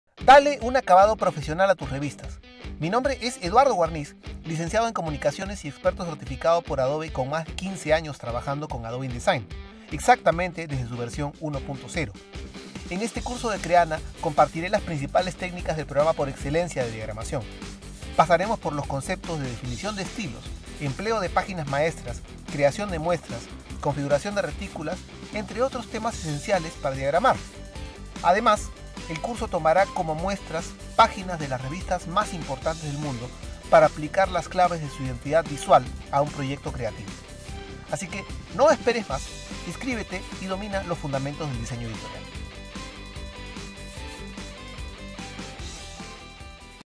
Curso de Diseño Editorial desde cero
El diseño editorial es uno de los artes con más ciencia involucrada en su haber. Enfocándose principalmente en la presentación correcta y pensada en la experiencia del usuario, toda diagramación hecha con Adobe InDesign debe de combinar altos niveles de calidad artística con funcionalidad editorial.